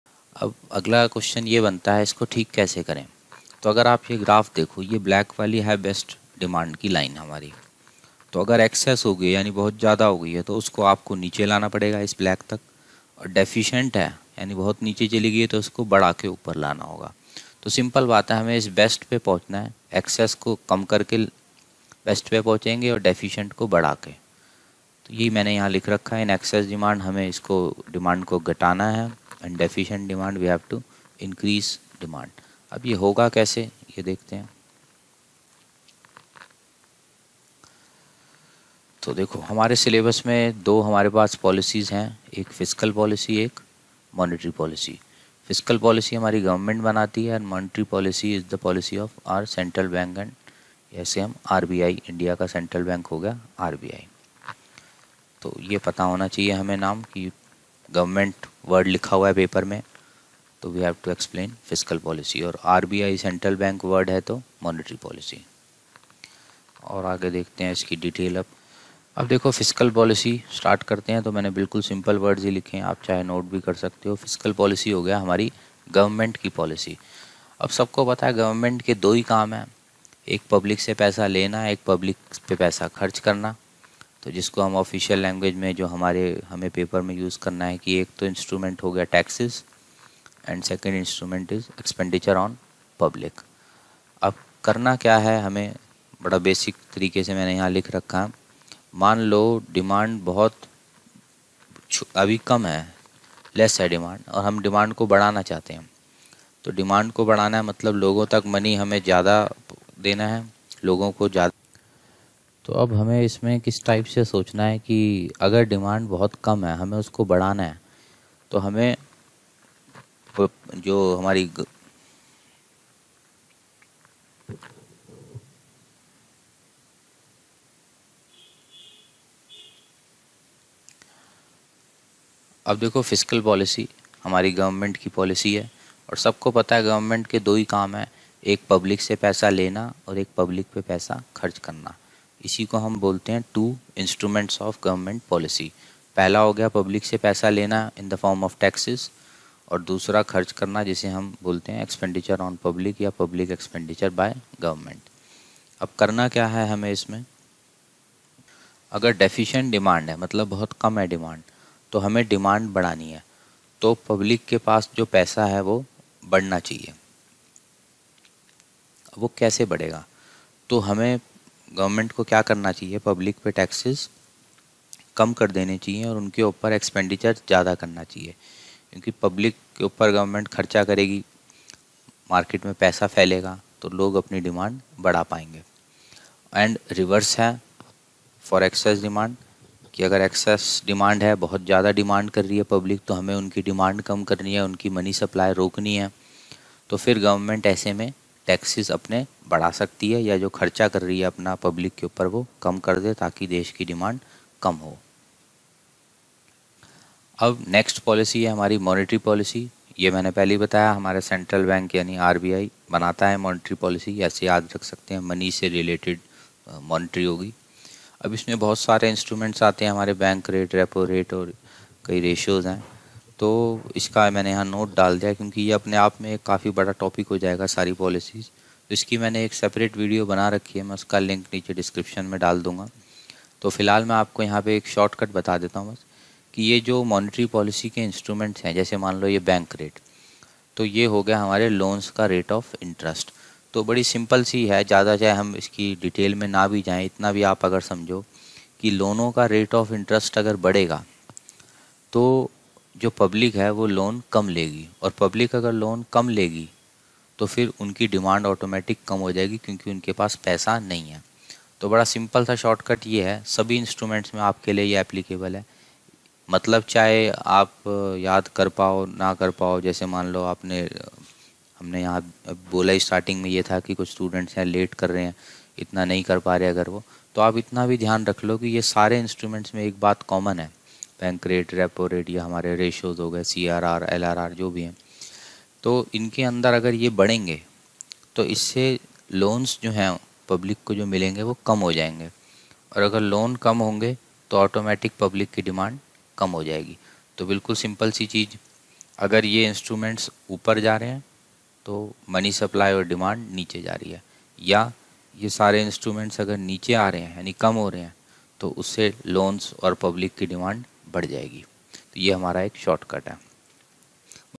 8. 100% Expected - FISCAL AND MONETARY POLICY - CBSE 2024 ECO
100% EXPECTED MACRO ECO QUESTIONS IN CBSE BOARD EXAM WITH 5 MINUTES SHORTCUT VIDEO OF EACH TOPIC